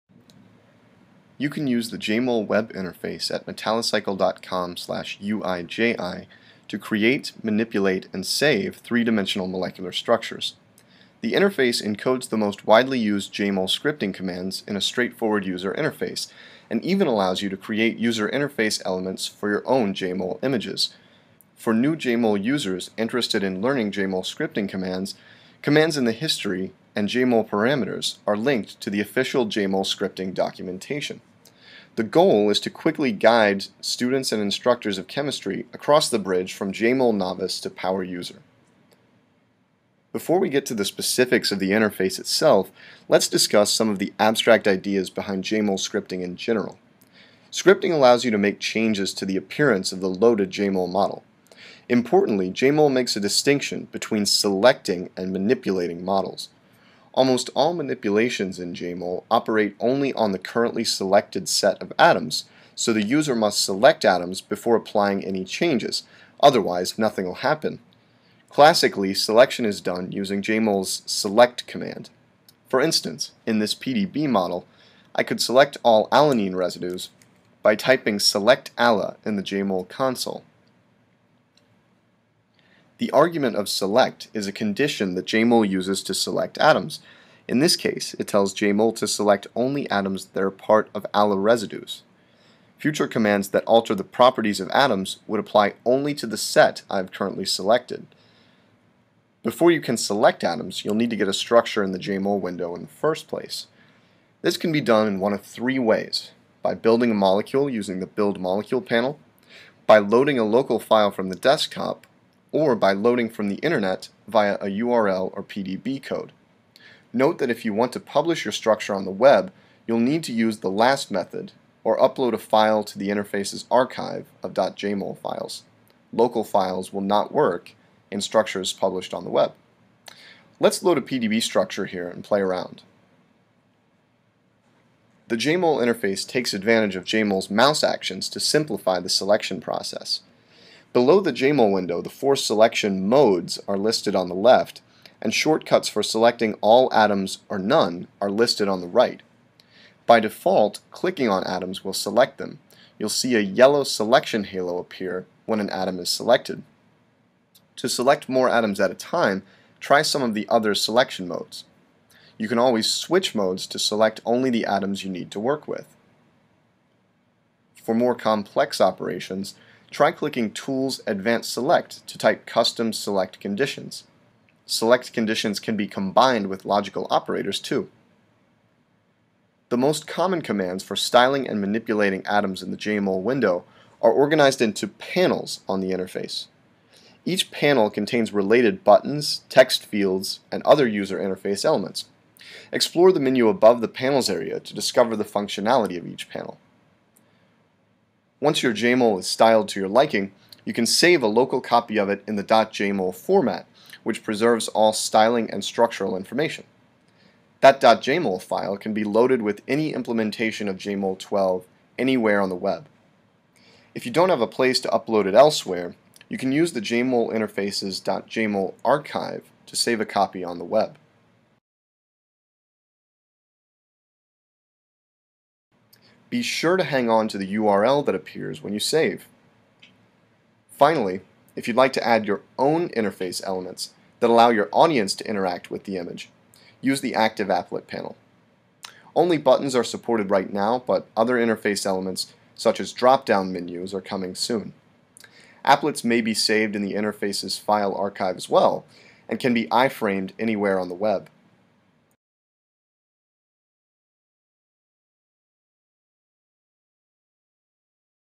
Jmol Interface 2: Three-dimensional Molecular Models Made Easy
I recently embarked on an epic (and badly needed) update of the University of Illinois Jmol Interface. If you're interested in building computational molecular models but don't want to learn Jmol scripting, give it a try